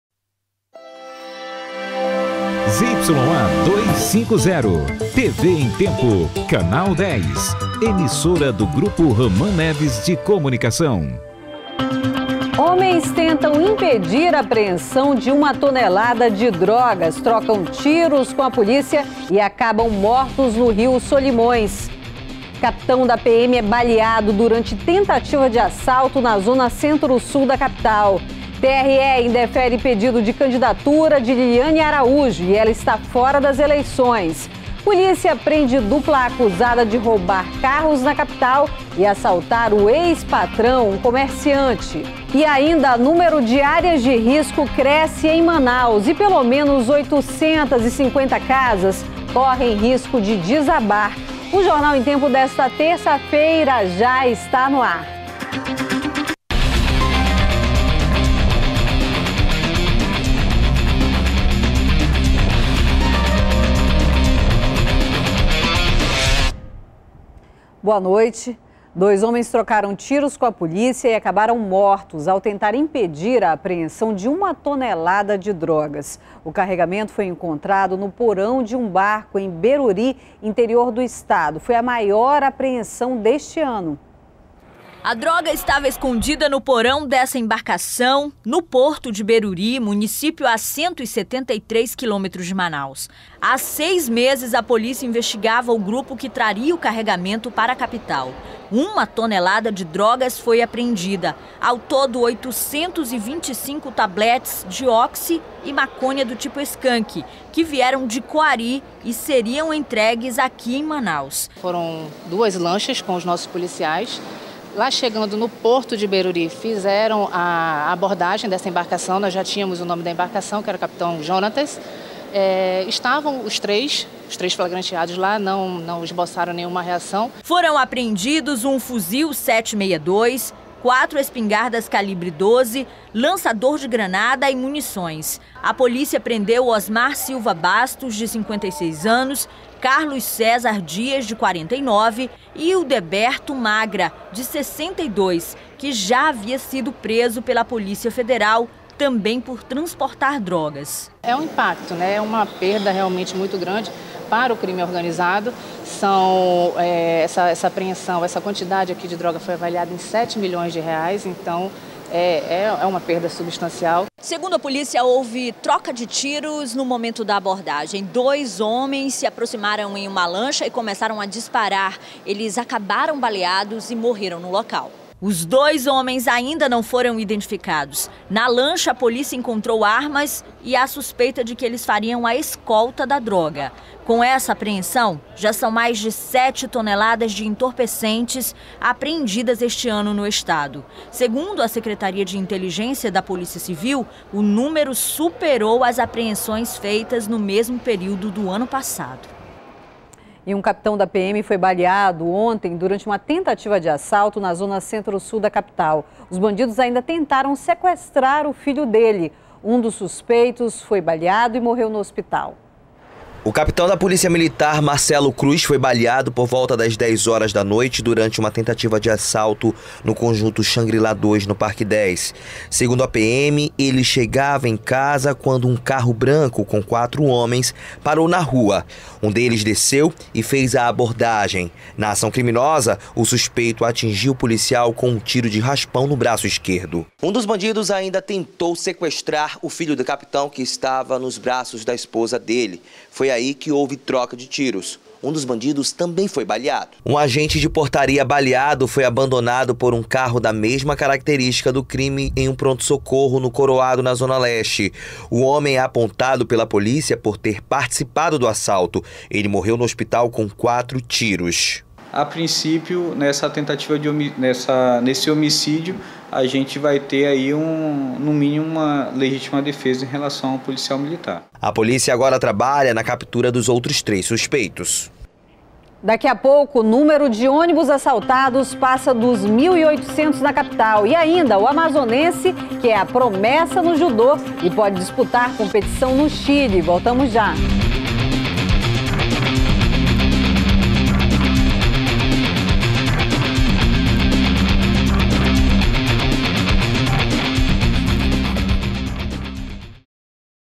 Polícia apreende 1 tonelada de droga no porão de embarcação em Beruri
01º BLOCO JORNAL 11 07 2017 - Polícia apreende 1 tonelada de droga no porão de embarcação em Beruri